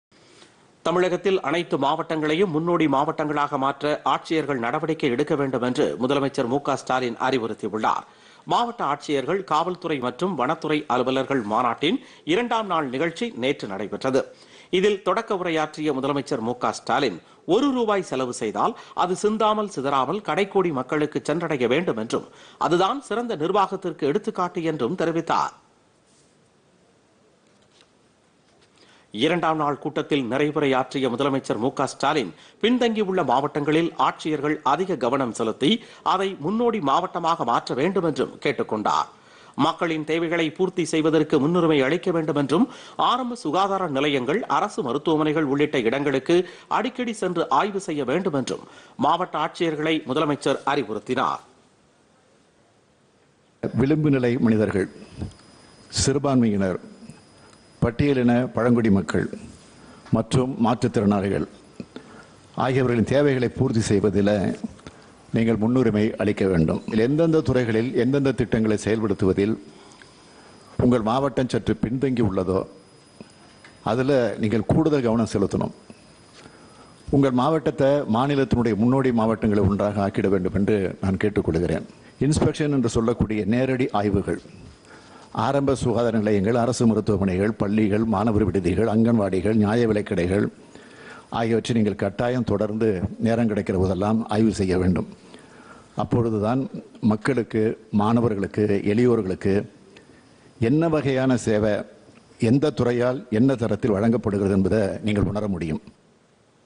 முன்னோடி மாவட்டங்களாக மாற்ற நடவடிக்கைPodhigaiTamilNews
தமிழகத்தில் அனைத்து மாவட்டங்களையும் முன்னோடி மாவட்டங்களாக மாற்ற ஆட்சியர்கள் நடவடிக்கை எடுக்கவேண்டும் -  முதலமைச்சர் மு க ஸ்டாலின் அறிவுறுத்தல்